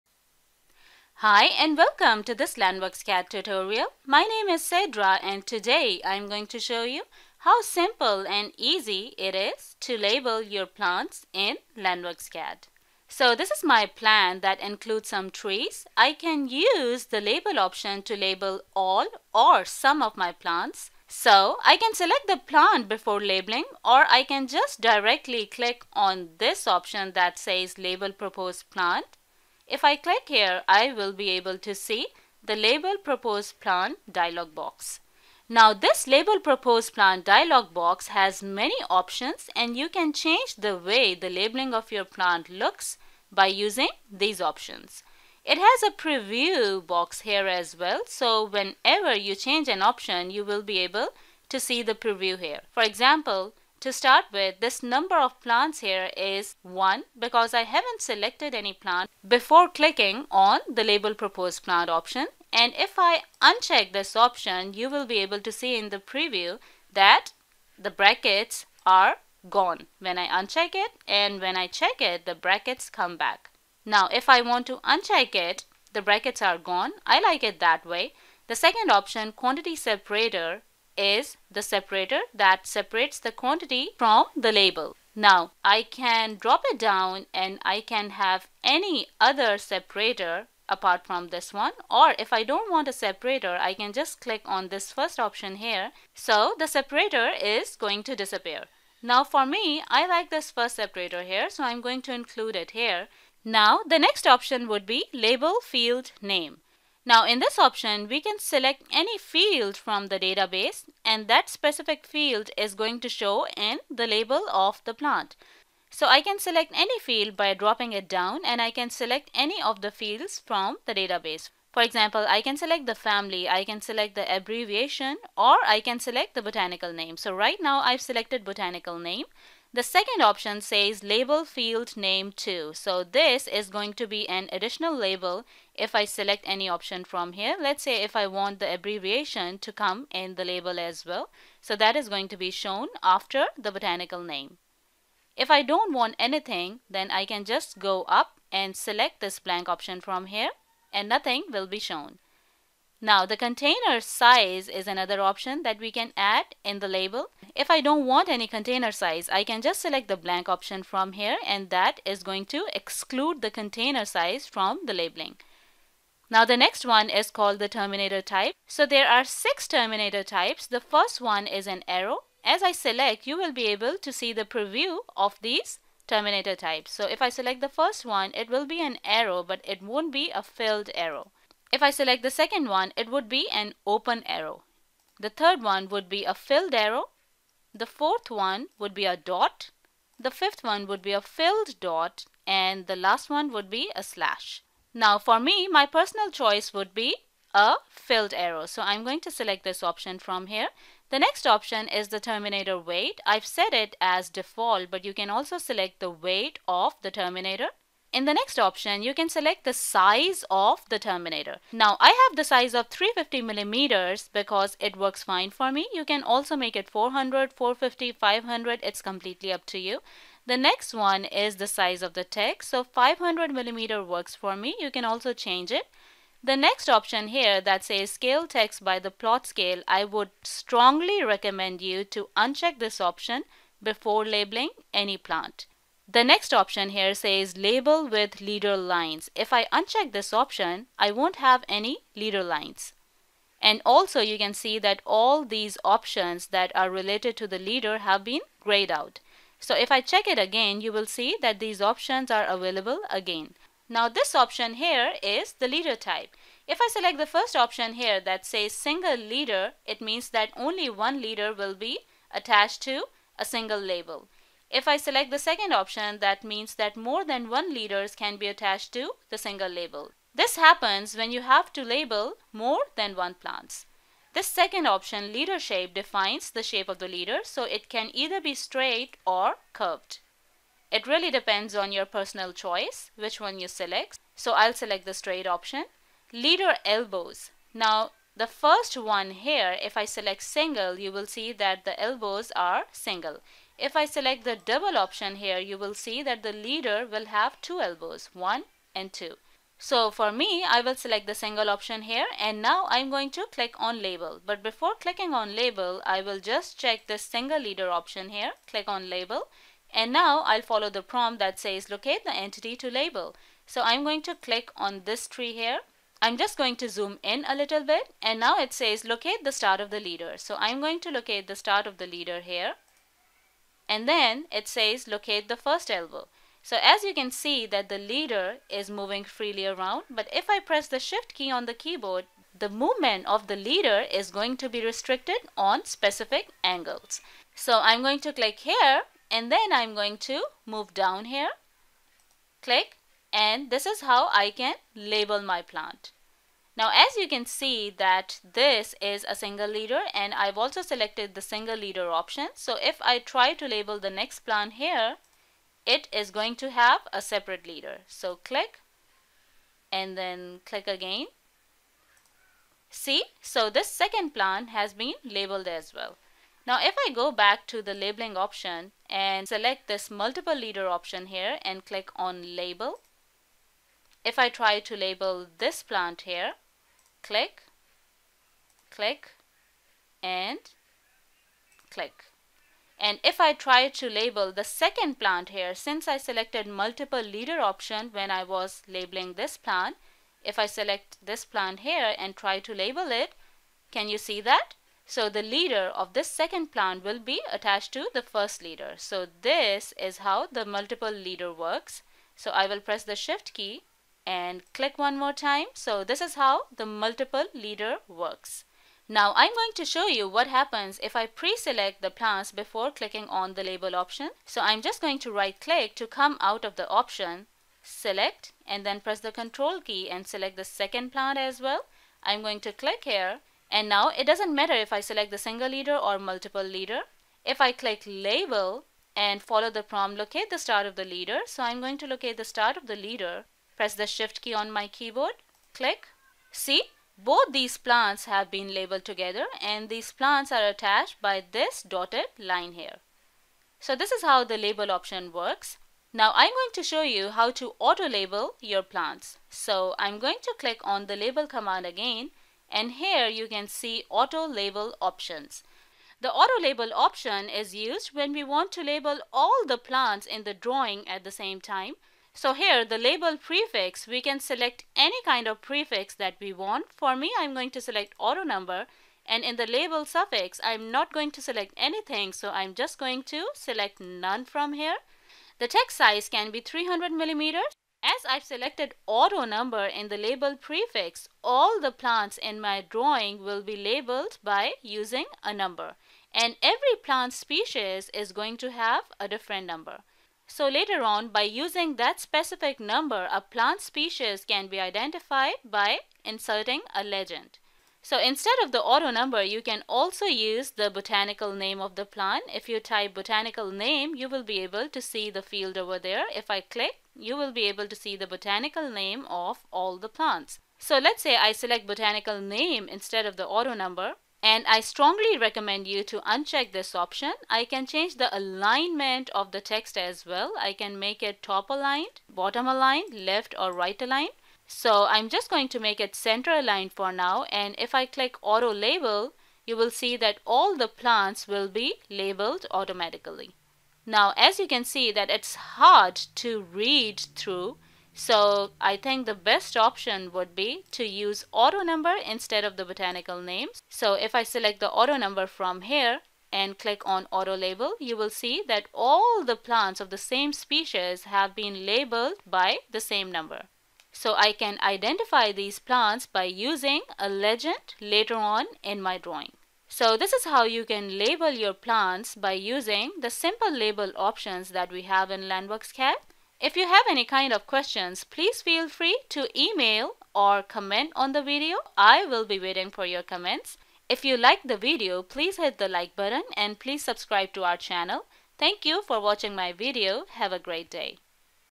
How to Label Plants in LANDWorksCAD v7
This video tutorial shows how to label plants in LANDWorksCAD and explains how to customize the labelling depending on your own needs.